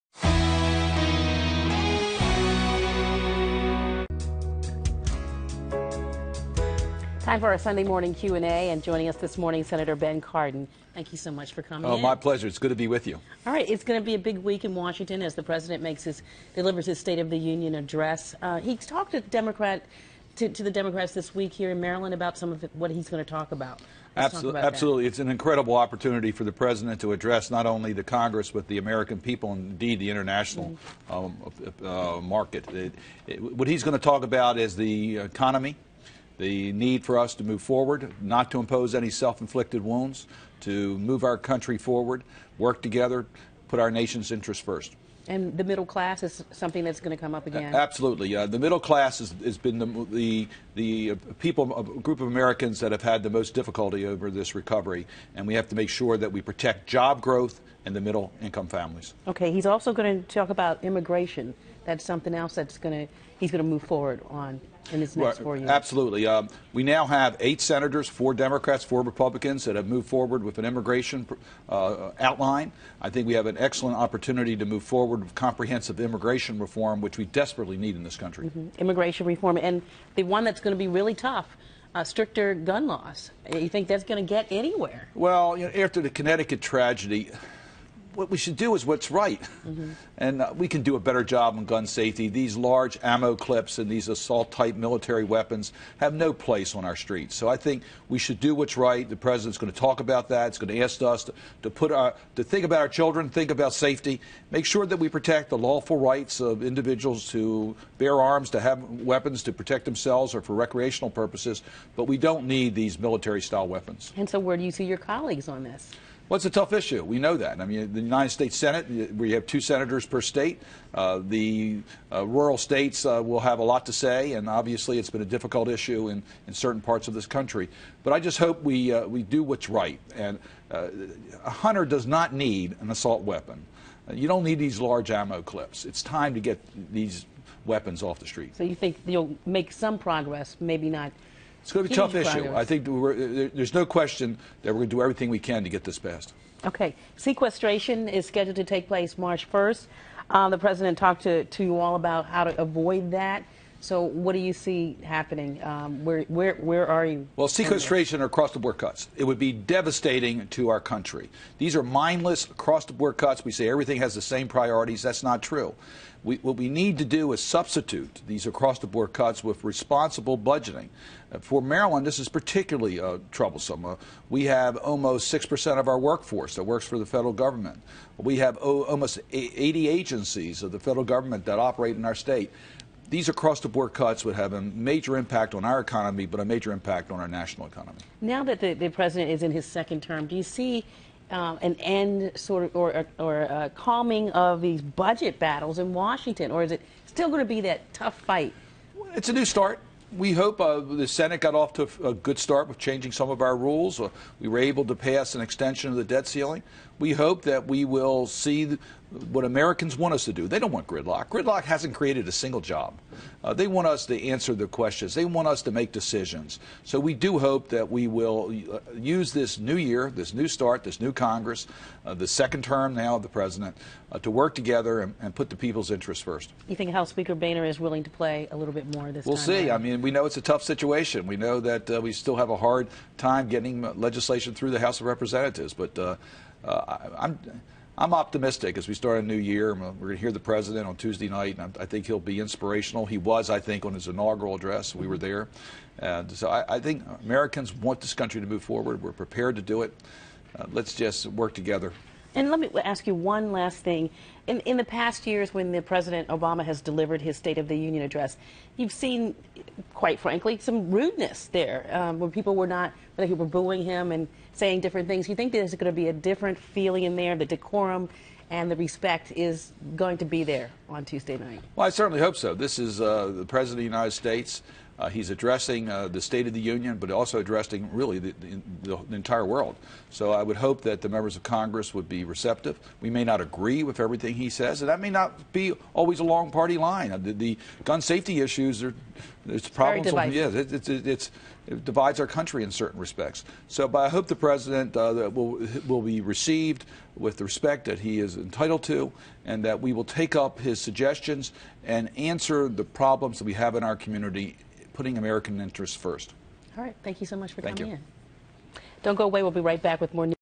U.S. Sen. Ben Cardin Sunday Q & A guest
U.S. Sen. Ben Cardin covers a range of issues, including immigration, gun laws and middle class families hit hard by the recession.